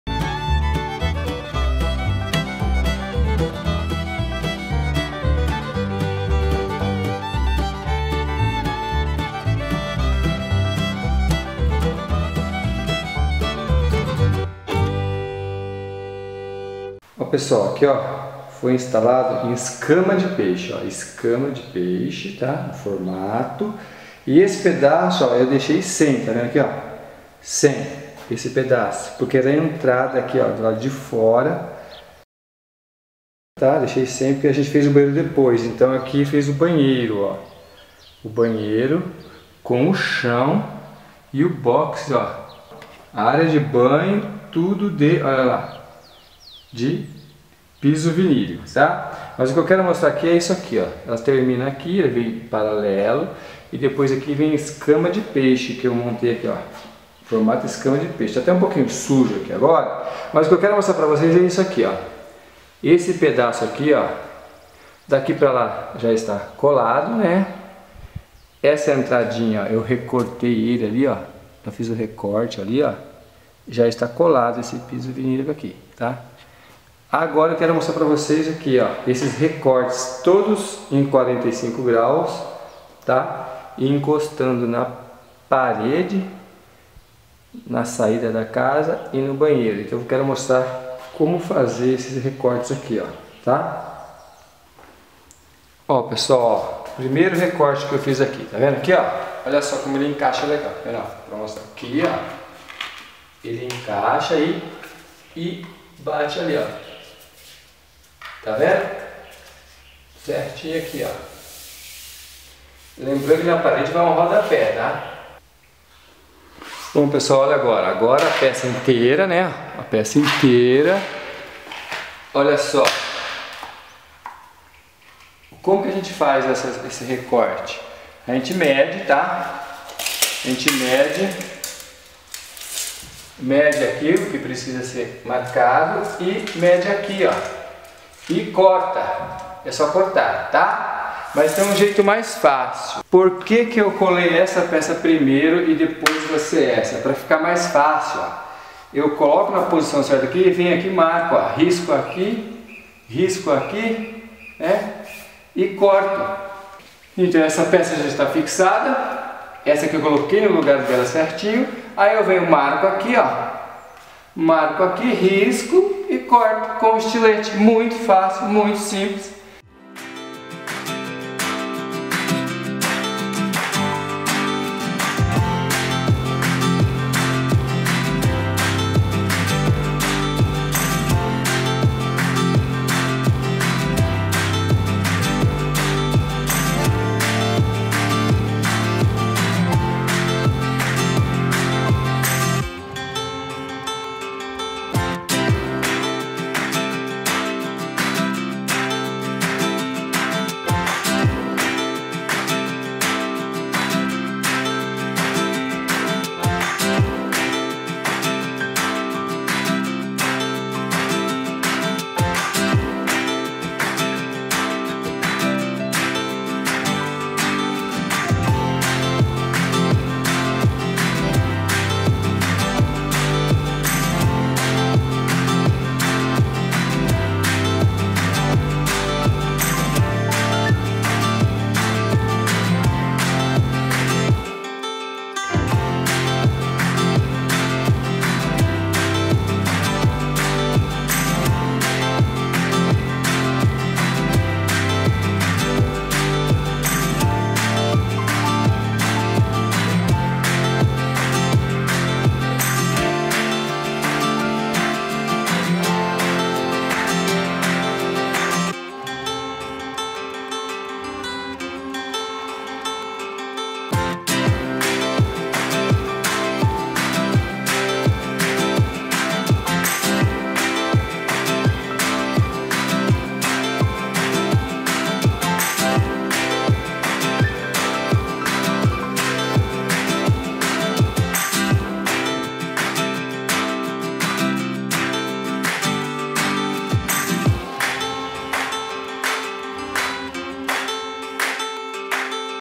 PISO VINÍLICO RECORTE PERFEITO
NESTE VÍDEO MOSTRO COMO CORTAR DE UMA FORMA FÁCIL O PISO VINÍLICO EM 45°. ESTE É O PISO DA MARCA VINILE , ACESSE